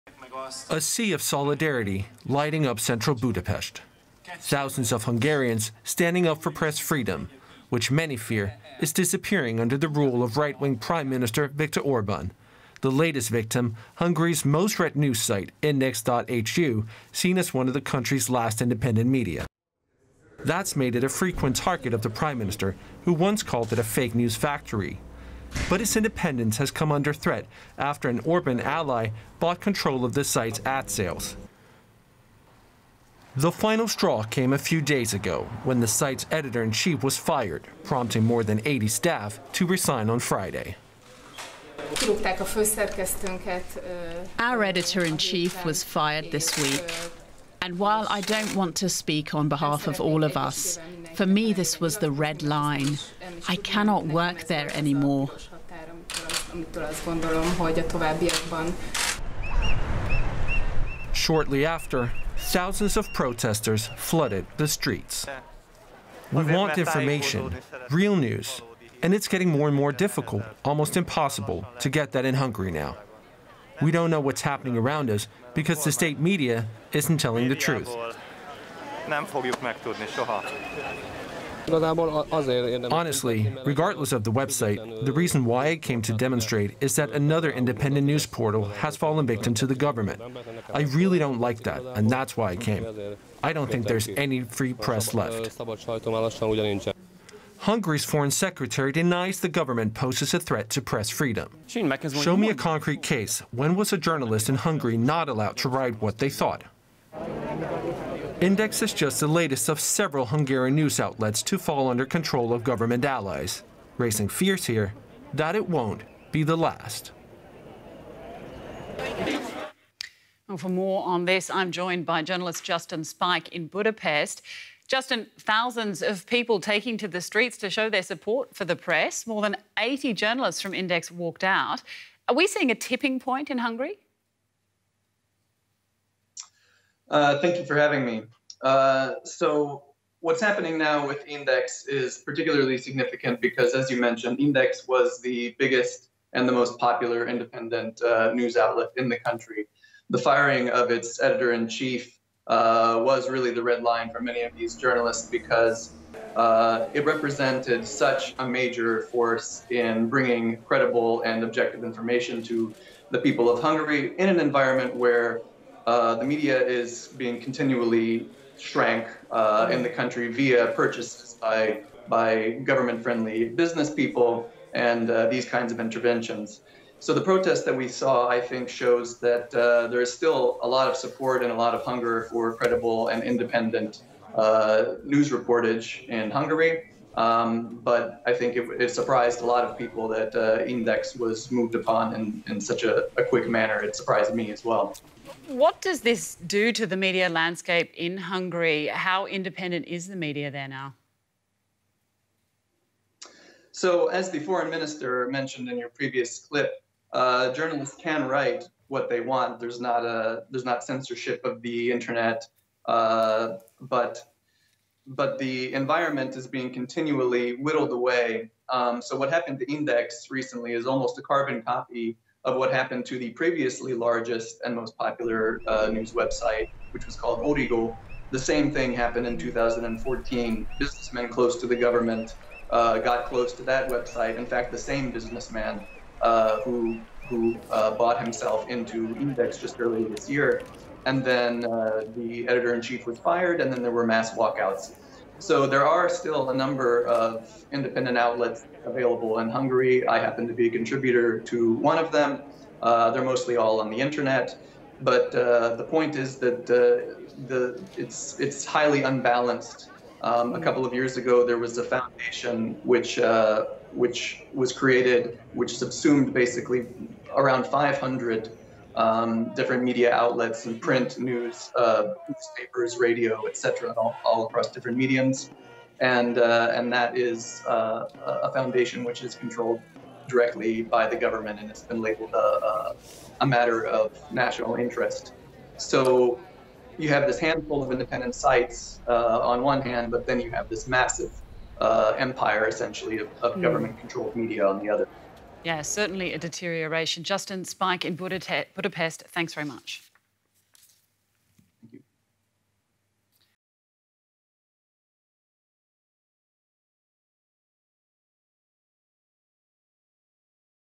Thousands in Hungary march for press freedom after 'Index' editor fired | DW News
Thousands of protesters marched through central Budapest in support of journalists at Hungary's most-read news site, Index. Dozens of journalists resigned over the sacking of its editor-in-chief earlier this week. His dismissal has raised concern that Prime Minister Viktor Orban's nationalist government is intensifying its effort to muzzle critical voices. Is press freedom in danger in Hungary?